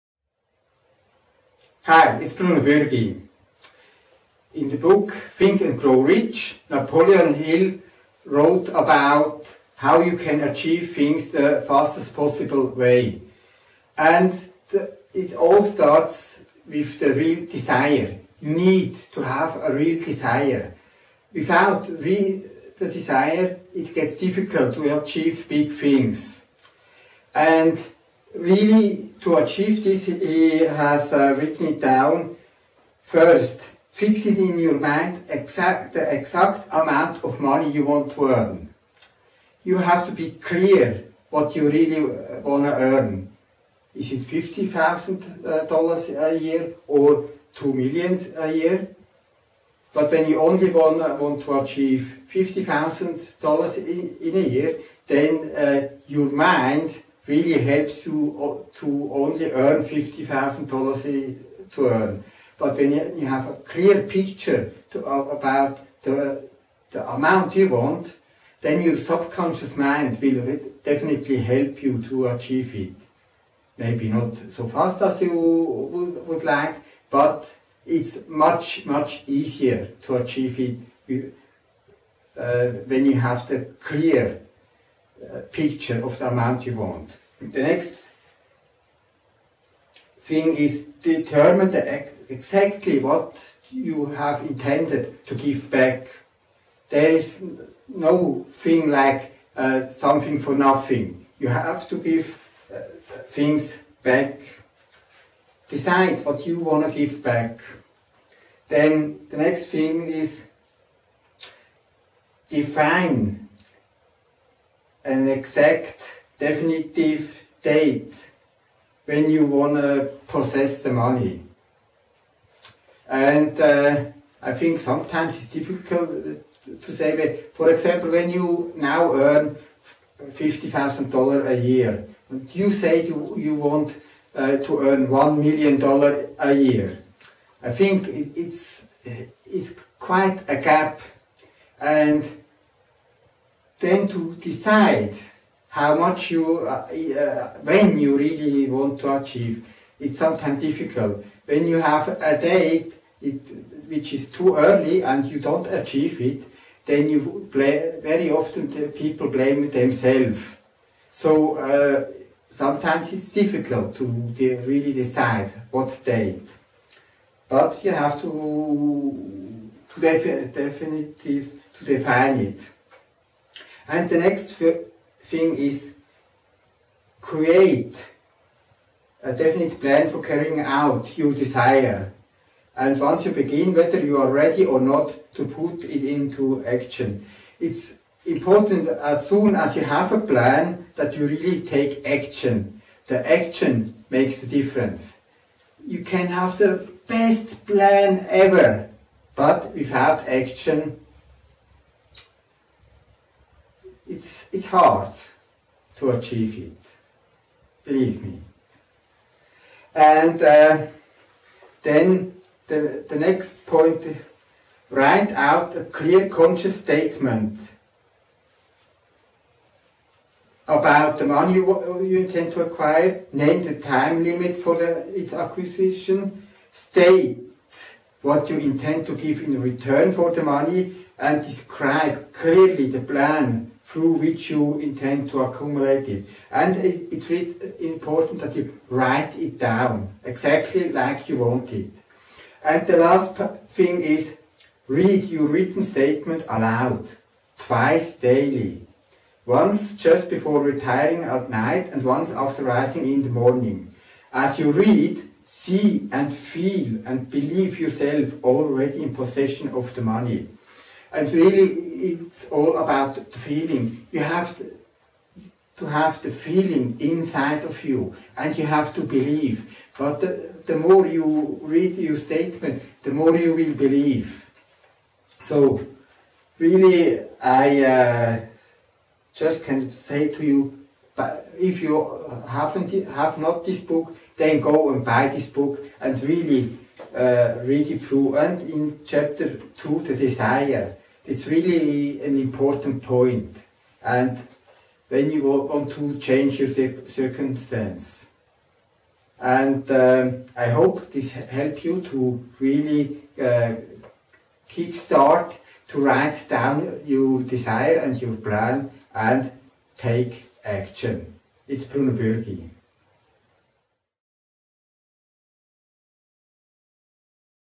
Desire
Desire - The Starting Point Of All Achievement

Desire is the second chapter in Napoleon Hill’s book „Think and Grow Rich“. He pointed out how to achieve things in the fastest possible way. Here are the six steps to it

1: Fix in your mind the exact amount of money you desire.
2: Determine exactly what you intend to give in return for the money you desire.
3: Establish a definite date when you intend to possess the money you desire.
4: Create a definite plan for carrying out your desire, and begin at once, whether you are ready or not, put this plan into action.
5: Write out a clear, concise statement of the amount of money you intend to acquire. State all points above.
6: Read your written statement aloud, twice daily, once just before retiring at night and once after rising in the morning.